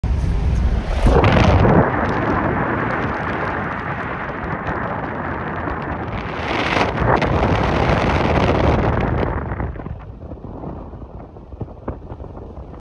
DA20 airplane emergency landing on the 95 freeway Las Vegas
Saw this plane on the side of the road this morning 01/07/2023 DA20 two passenger airplane